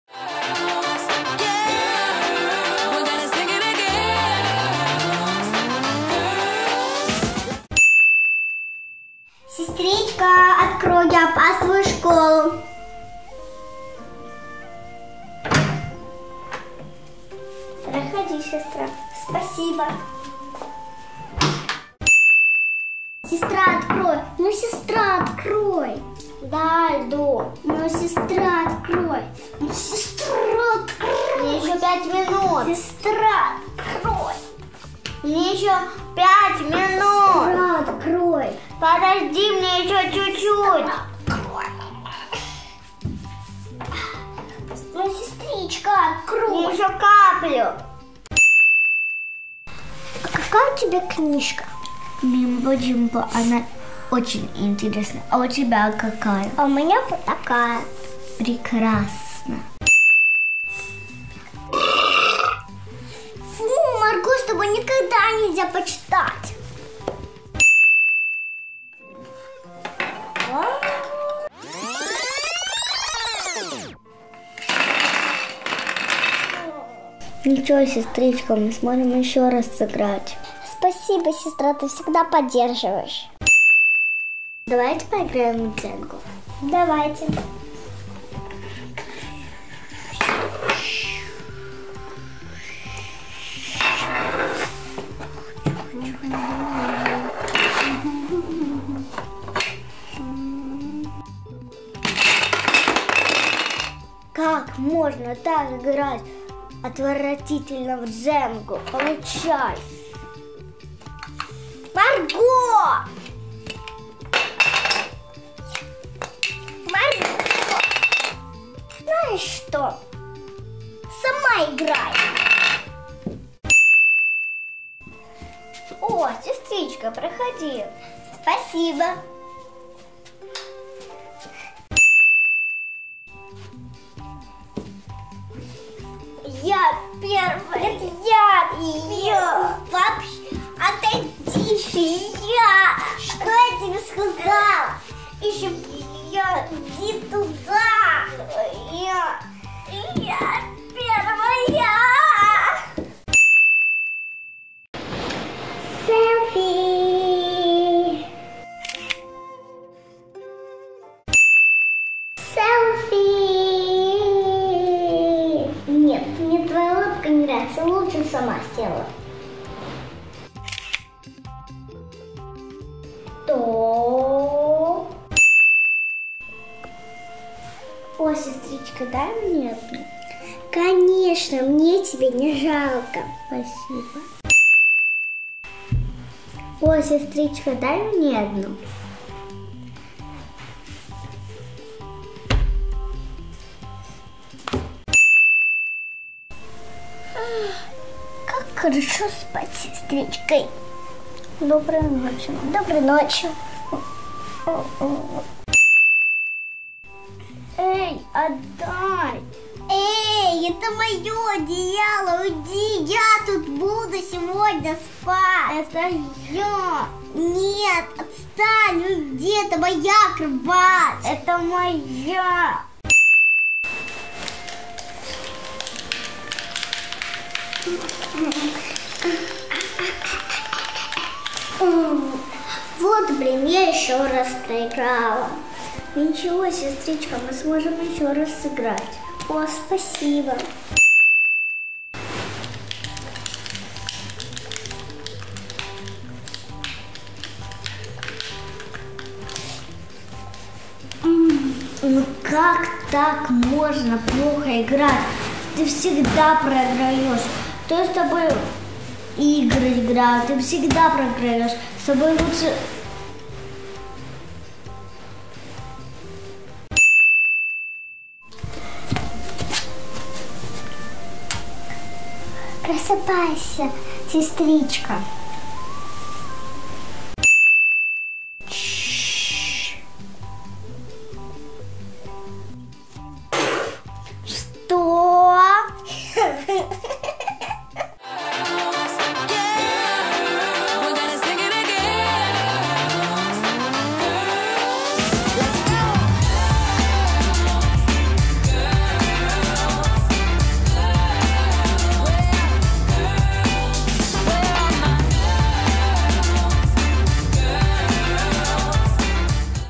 ДВЕ СЕСТРЫ. Ожидание и Реальность лучших сестричек
Короткие истории из жизни сестер. ожидание и реальность! как на самом деле живут сестры)
Посмотрите на разницу как живут 2 сестры в реальном мире и как мы видем в ожиданиях))